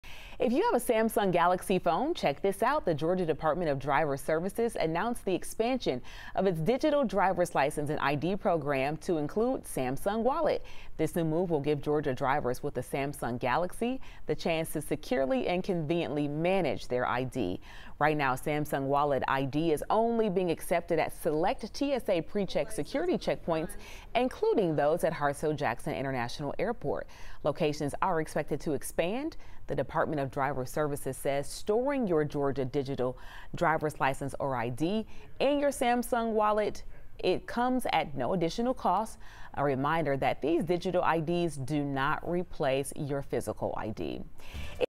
Georgia DDS adds Samsung Wallet to digital driver's license, ID program
Samsung Wallet users will now be able to add their ID to their digital wallet on their phones for convenience.